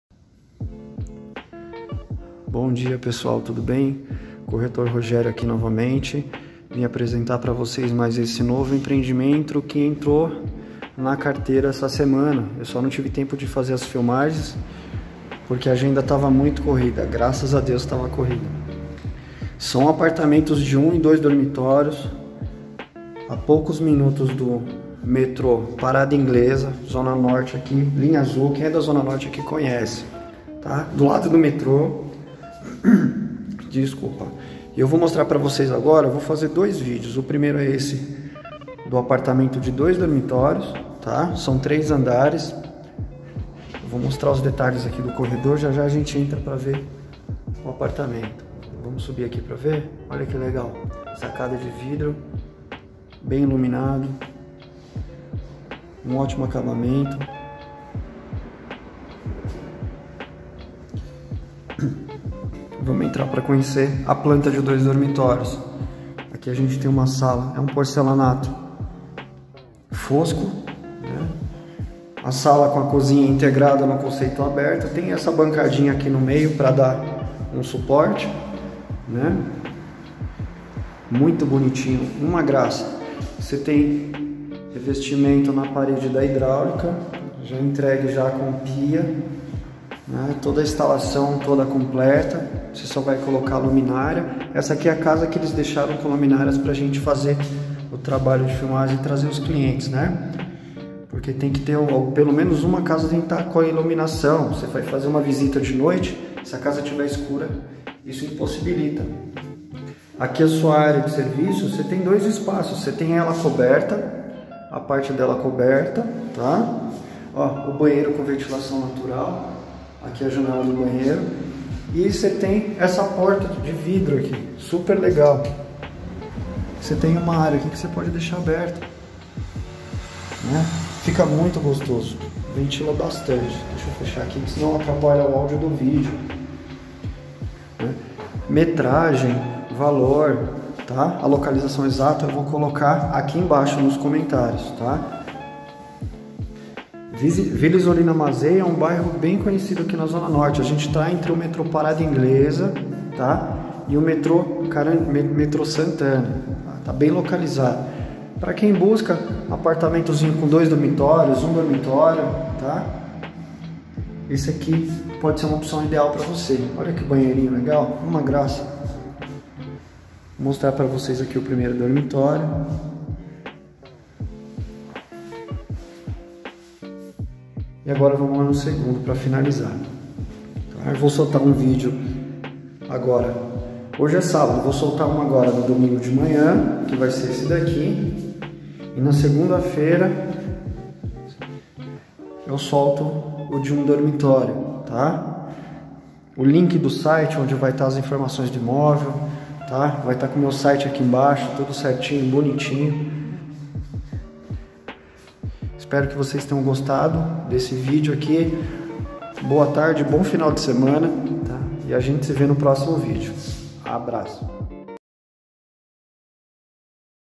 Apartamento a venda na Parada Inglesa.
imoveis Condomínio com 15 unidades no total.
Com 1 ou 2 Dorms, sem vaga de garagem.
R$ unidades com preço médio 270 mil